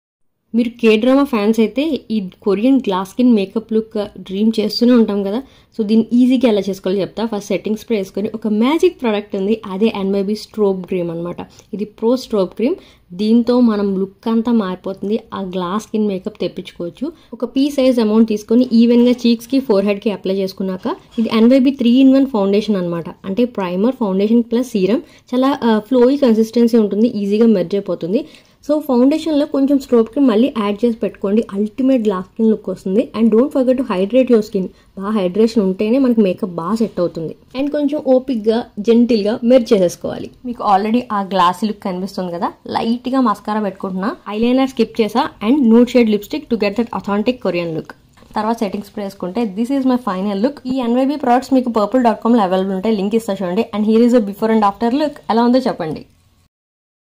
Viral Korean Glass Skin Makeup
Make my favourite products s part of your makeup kit:

NY Bae 3 in 1 foundation Cool Cashew 04

Ny Bae The Big Apple Of My eyes kajal| Black

NY Bae 5 in 1 Lipstick |Nude Moods 02

NY Bae Truly Matte Liquid Eyeliner Twin Pack - Brave Blue and Bold Black